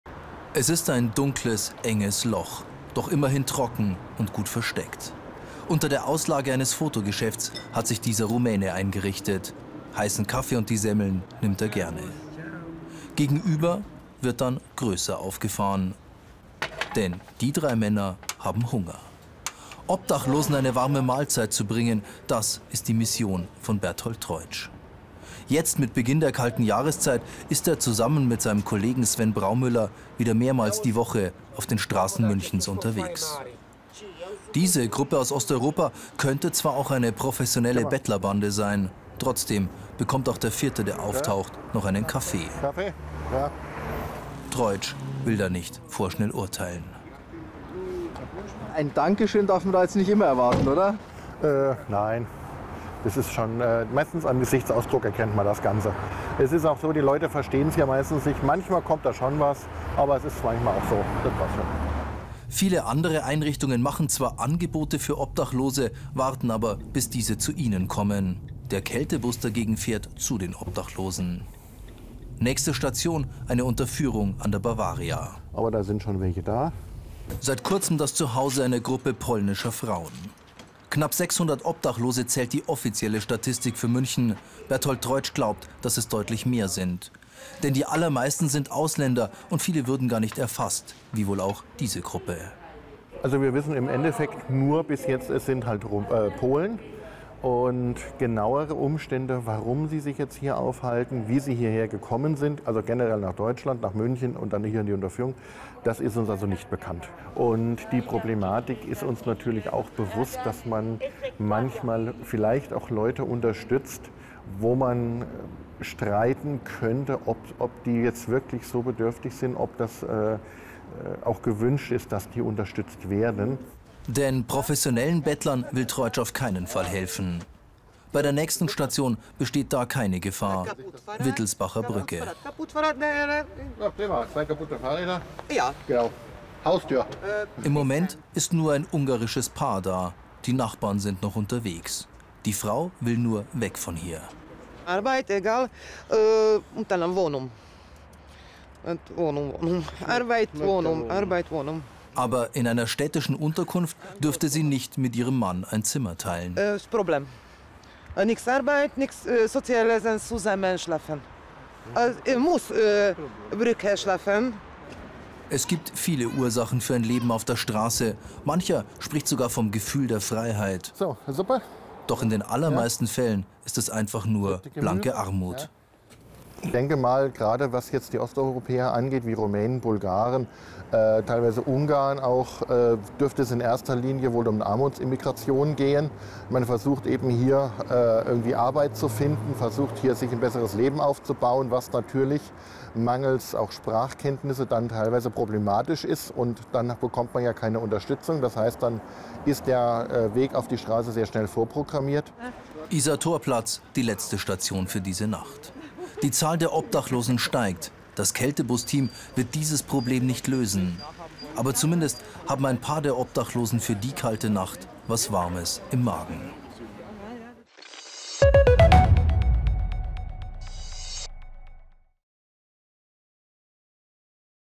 Kältebus versorgt Obdachlose in München | BR24
Seit ein paar Wochen verändert sich sichtbar das Bild der Münchner Innenstadt Nacht für Nacht. In vielen Hauseingängen zwischen Stachus und Gärtnerplatz schlagen Menschen ihr Nachtlager auf. Eine Reportage aus der Nacht.